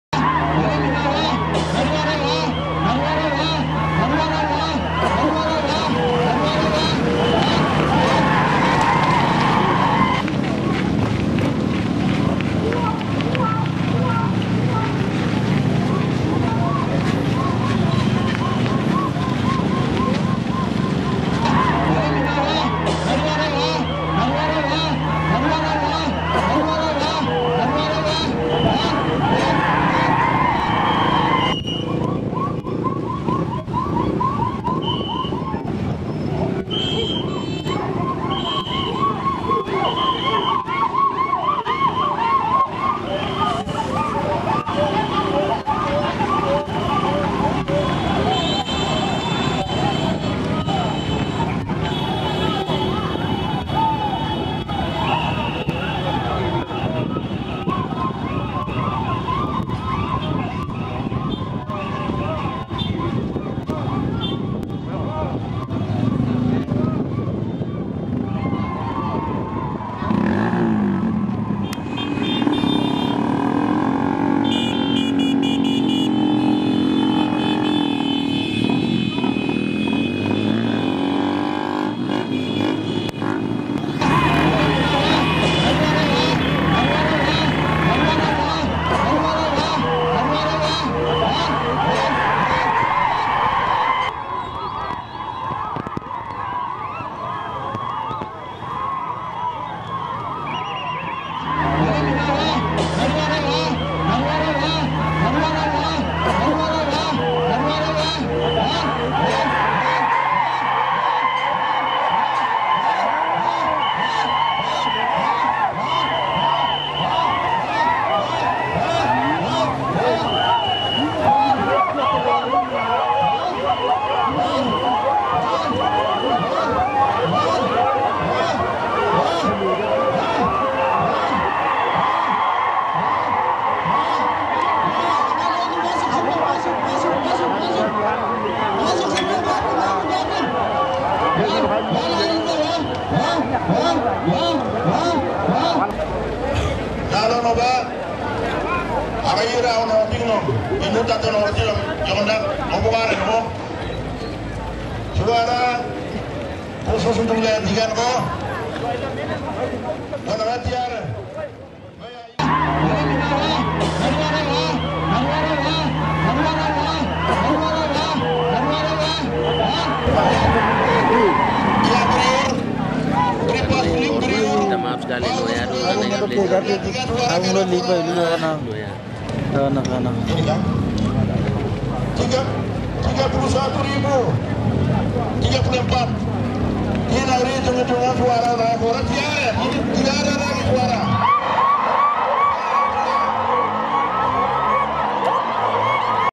Wow kayak mau perang, bikin takut! Massa Kampanye Akbar BRIPAS Kab. Lanny Jaya Papua 2017
video baru massa kampanye Akbar Calon Bupati Kab. Lanny Jaya, BRIUR WENDA, S. PD.,M.KP dan Calon wakil bupati PAULUS KOGOYA, S. SOS saat kampanye di ibu kota Kabupaten Lanny Jaya, Provinsi Papua. Pilkada serentak tahun2017.